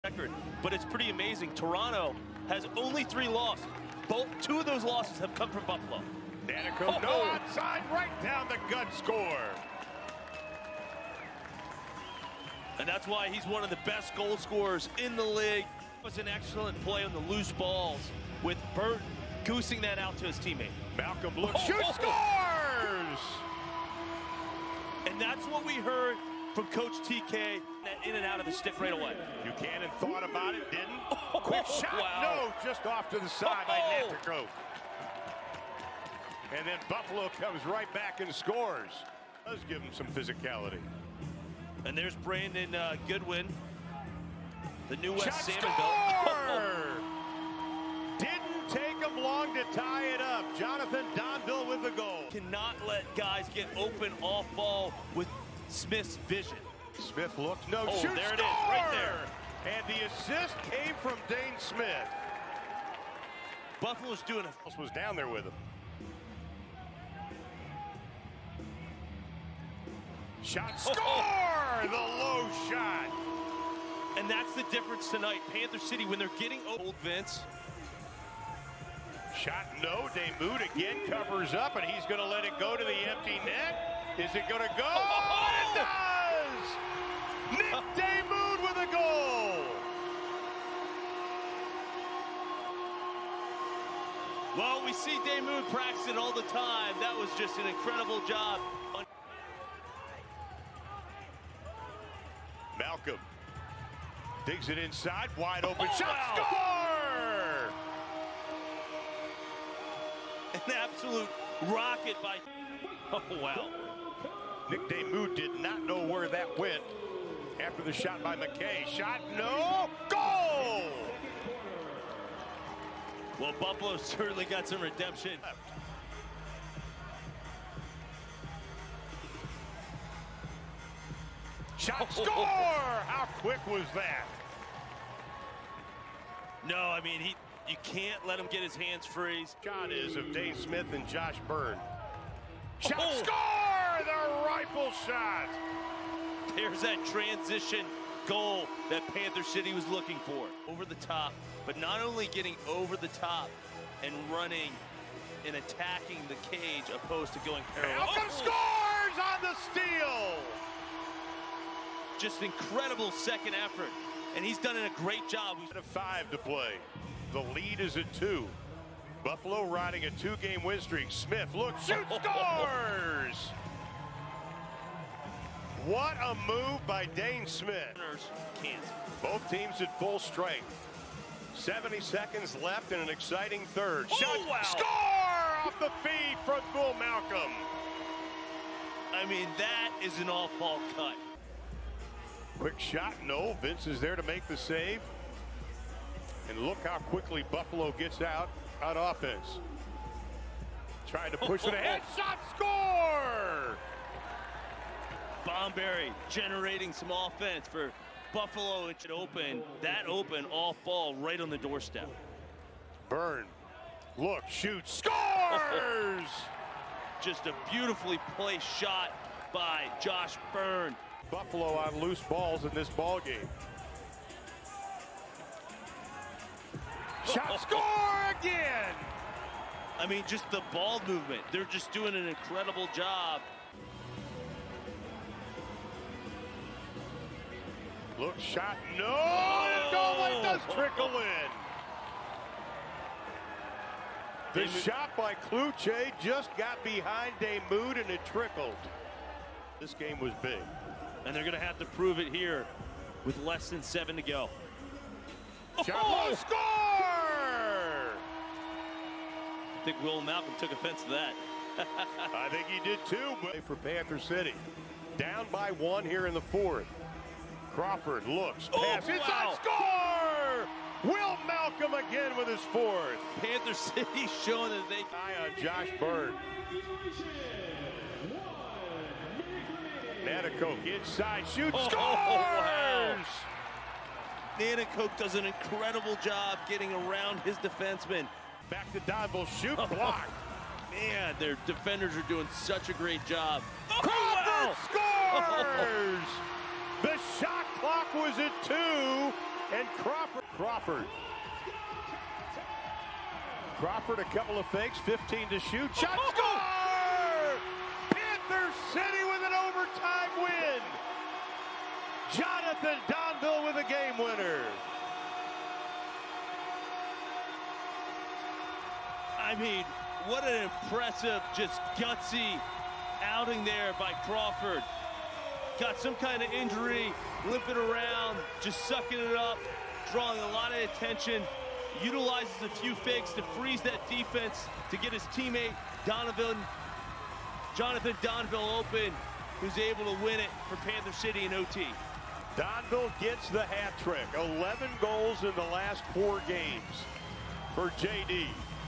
Full Game Highlights | Buffalo Bandits vs Panther City Lacrosse Club
Full highlights from Panther City's 12-11 OT win over the Buffalo Bandits.